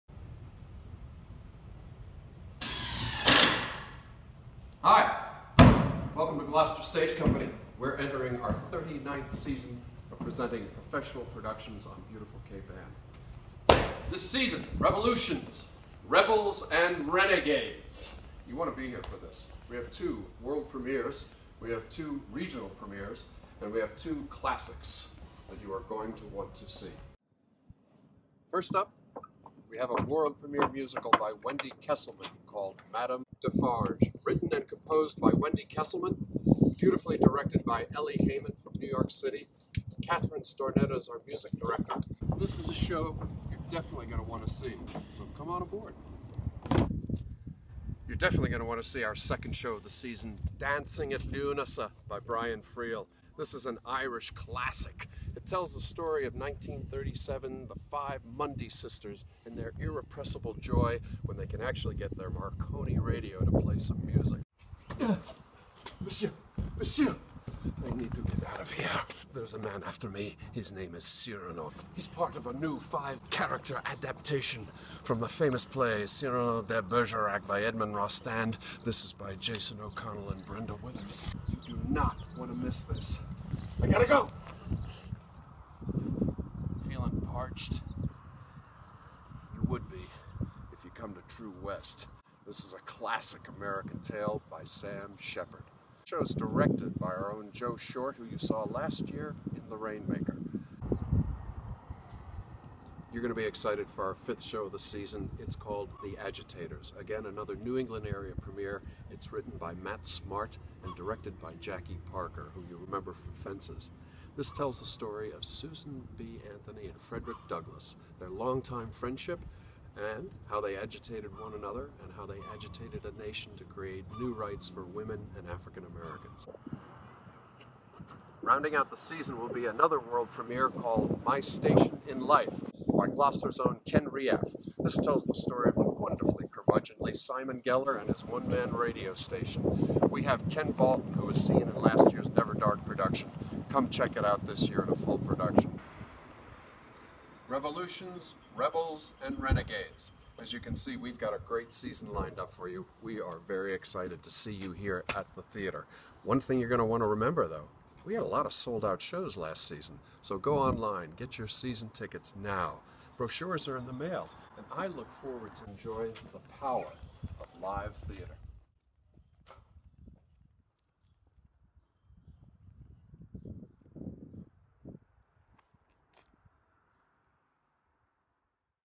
2018 Season Welcome
Bob Walsh takes us through the 2018 Season. Visit Gloucester Stage online at gloucesterstage.com